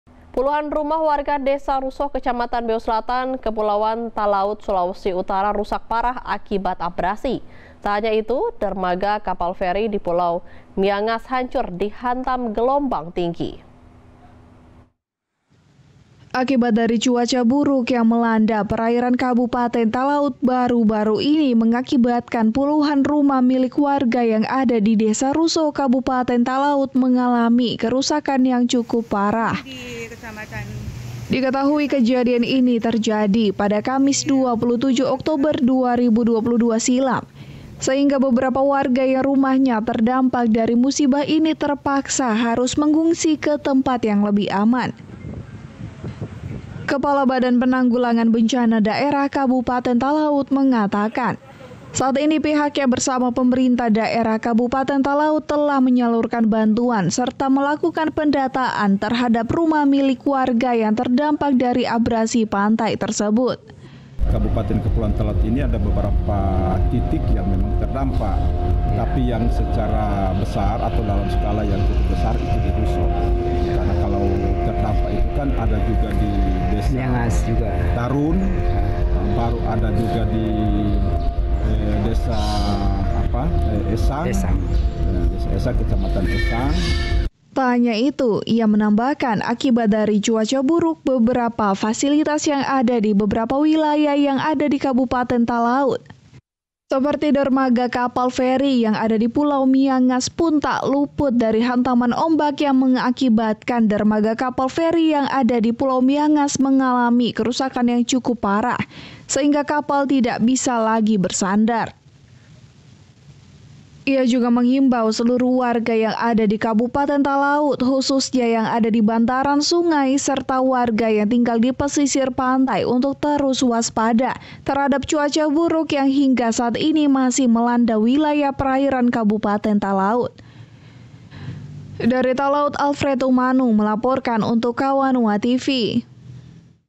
Abrasi dan Gelombang Tinggi Rusak Rumah dan Dermaga
Puluhan rumah warga Desa Rusoh, Kecamatan Beo Selatan, Kepulauan Talaud, Sulawesi Utara rusak parah akibat abrasi. Tak hanya itu, Dermaga Kapal Ferry di Pulau Miangas hancur dihantam gelombang tinggi.